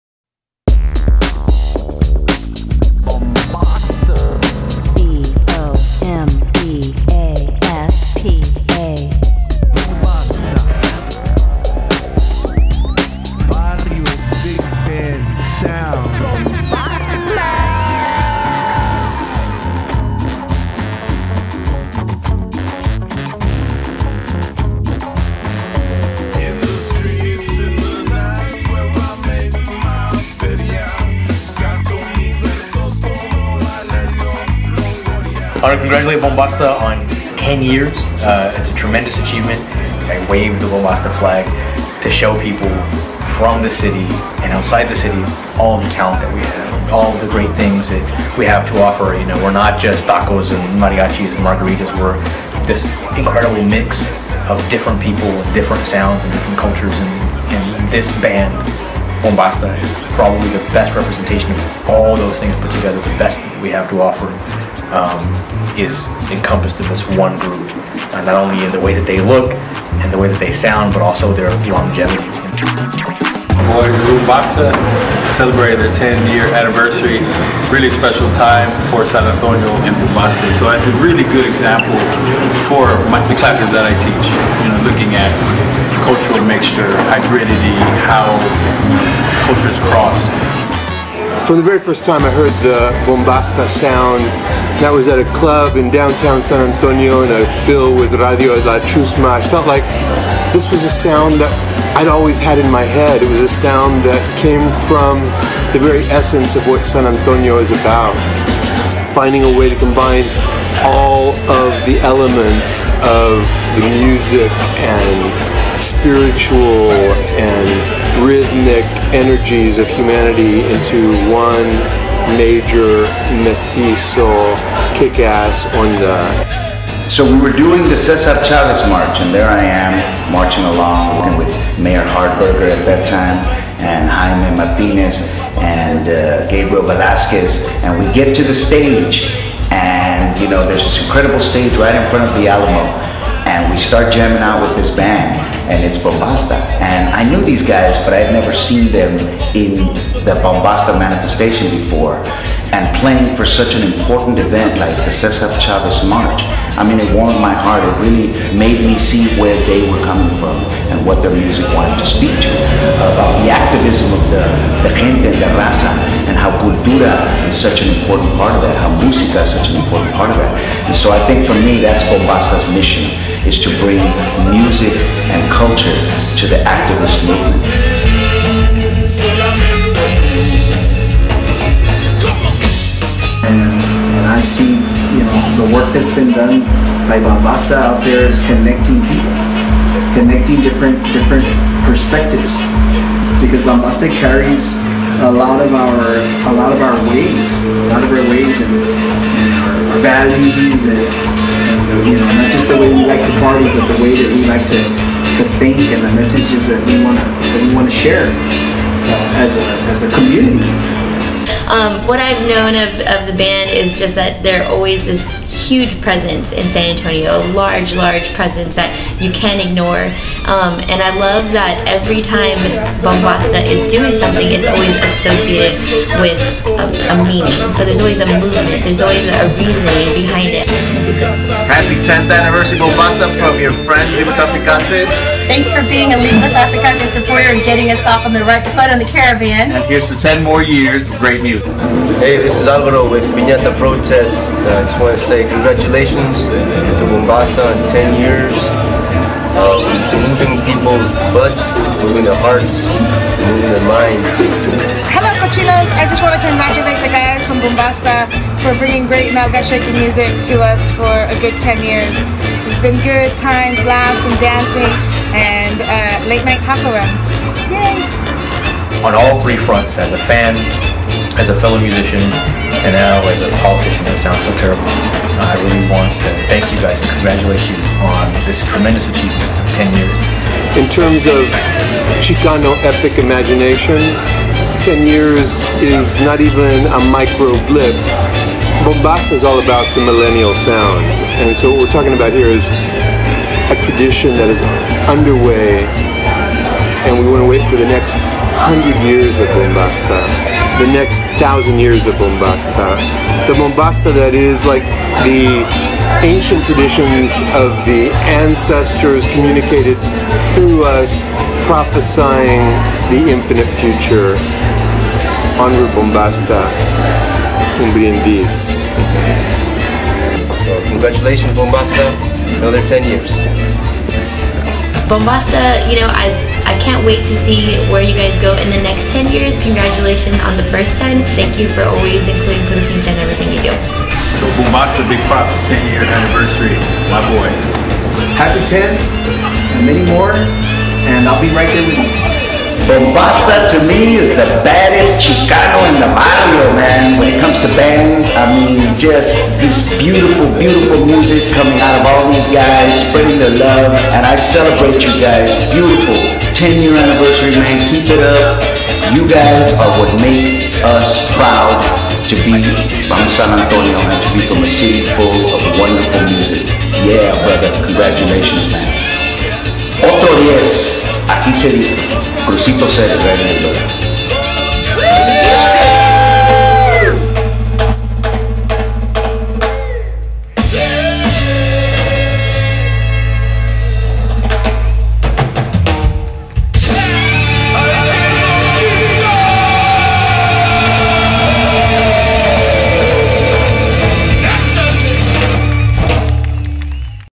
BOMBASTA: Ten Years Strong
Celebrating 10 Years of San Anto's 'Barrio Big Band'- BOMBASTA!
Kind words shared by Bombasta friends: Diego Bernal, John Phillip Santos, Jesse Borrego, Mexican Stepgrandfather, Puro Pinche, Librotraficantes, Alvaro Del Norte & Maricela Olguin
Produced by Robb Garcia